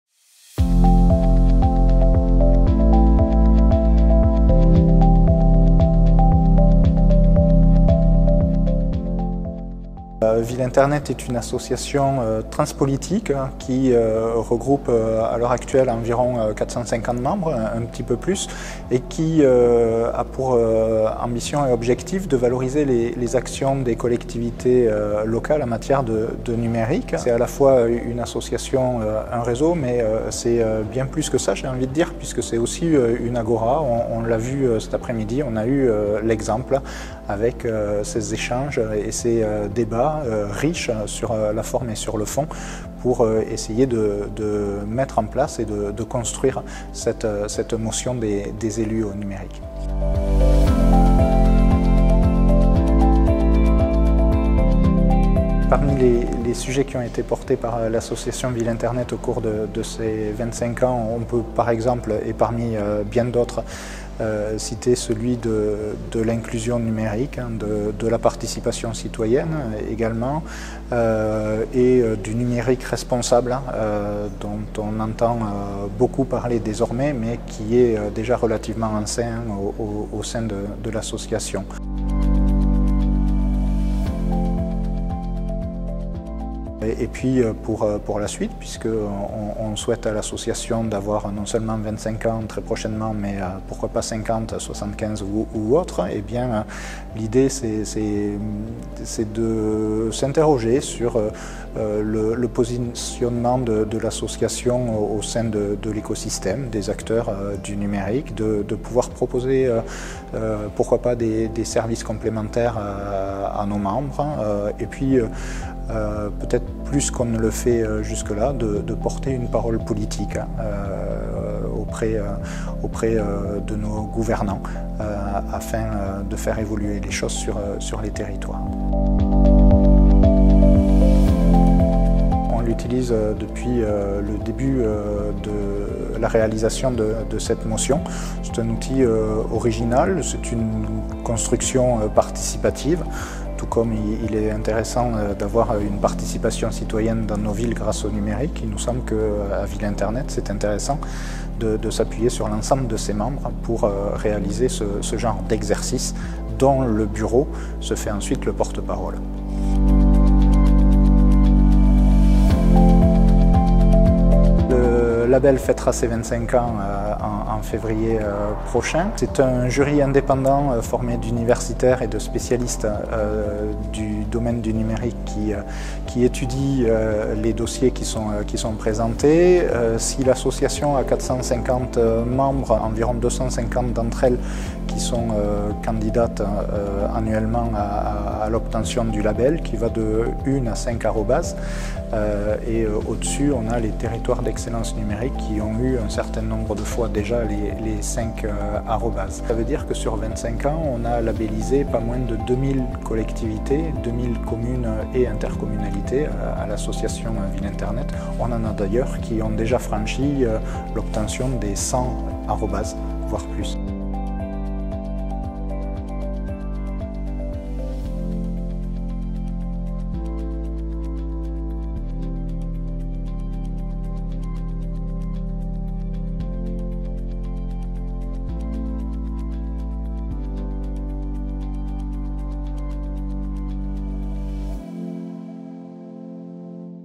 Mathieu VIDAL - Les élu·es qui font l'internet citoyen
[LES ELU·ES QUI FONT L’INTERNET CITOYEN]
25ème Edition du Label Villes Internet
Mathieu VIDAL, Président de l’Association Villes Internet
👉 Son rôle, ses sujets, ses ambitions, le Label
👉 Nous célébrerons la 25ème cérémonie du Label, le 8 février 2024 qui se déroulera au 101 rue de l’Université PARIS 7e.